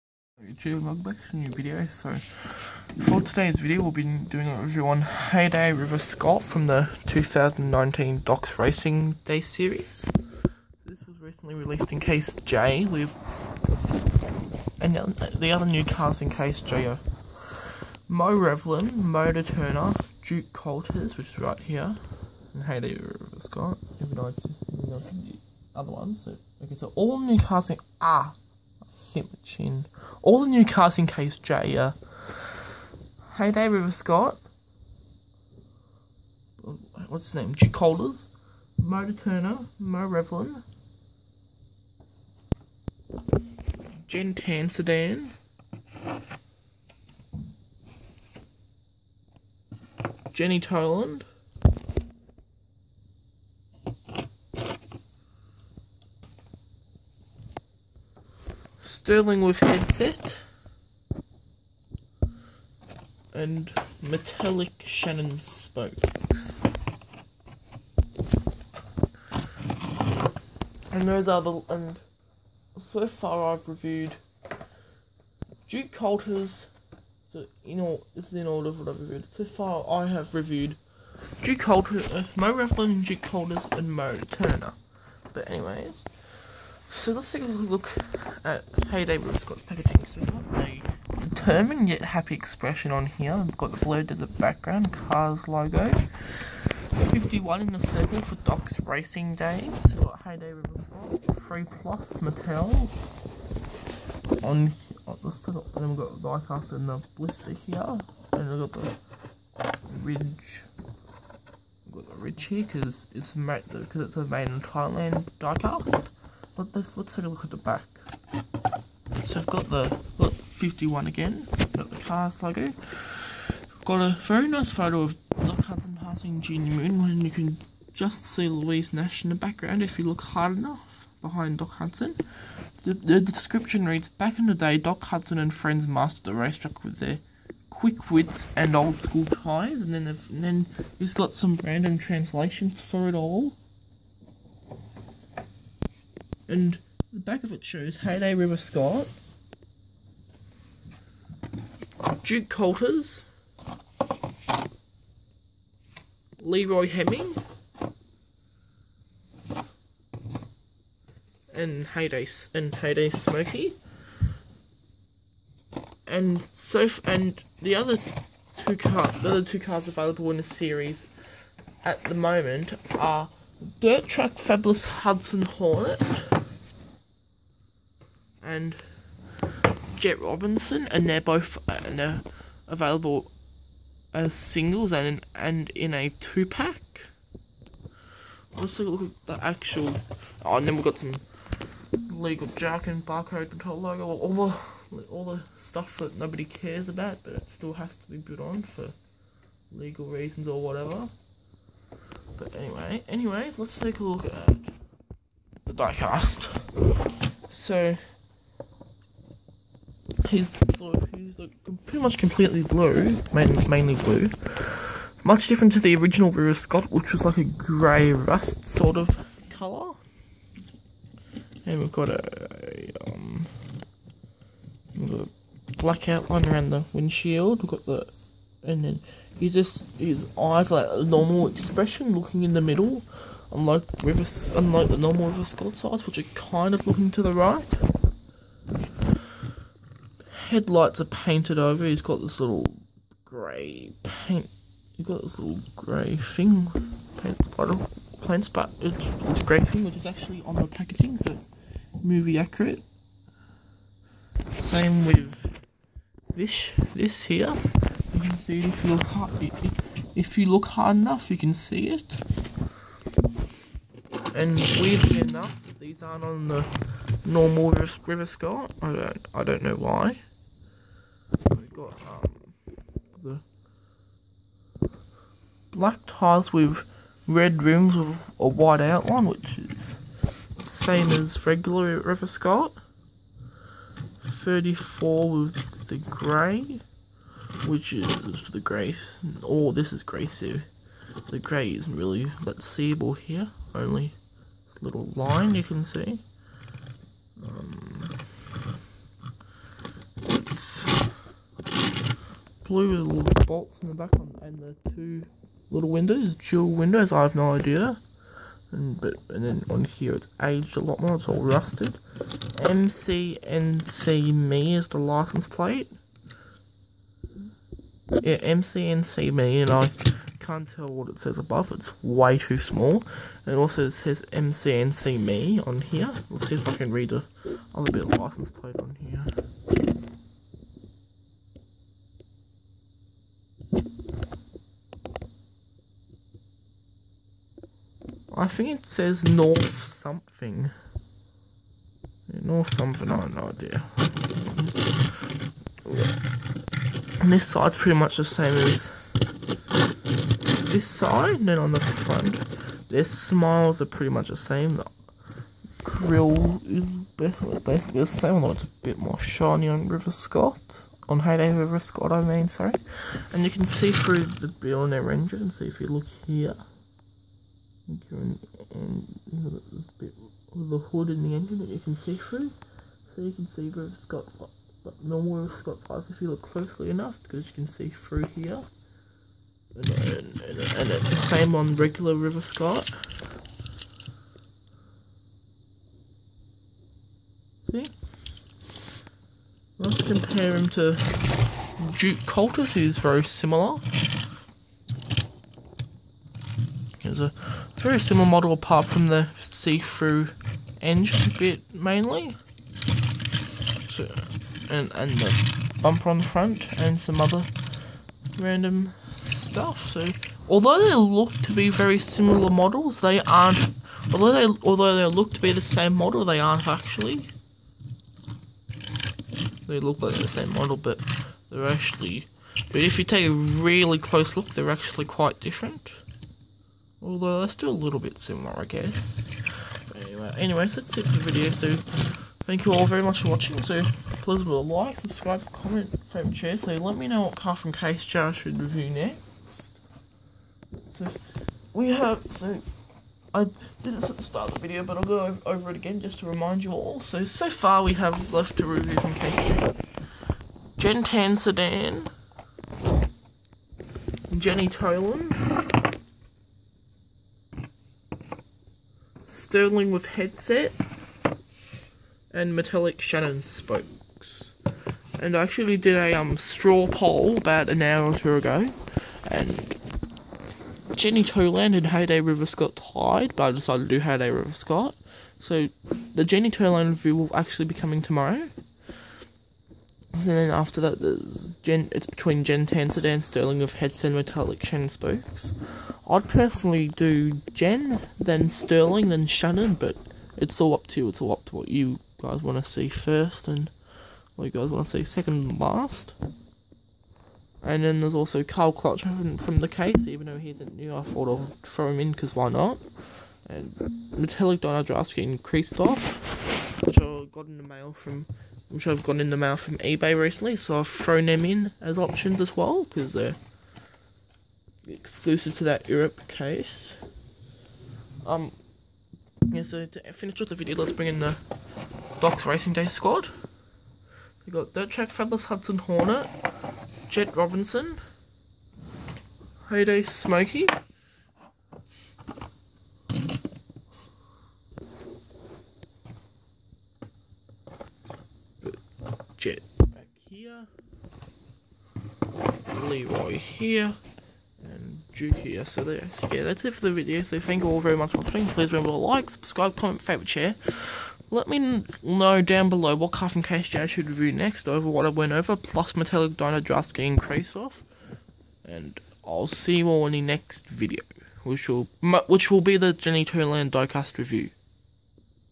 Cars 3 Diecast Review: Heyday River Scott
Today, I review Heyday River Scott! River Scott appears in his Heyday paint job in the Thomasville flashback scene centering on Doc Hudson and Leroy Heming. River goes on to appear with his normal paintjob in Thomasville, Radiator Springs and the Florida 500. Heyday River Scott has been released once, in the 2019 Doc's Racing Days series. He was made in Thailand.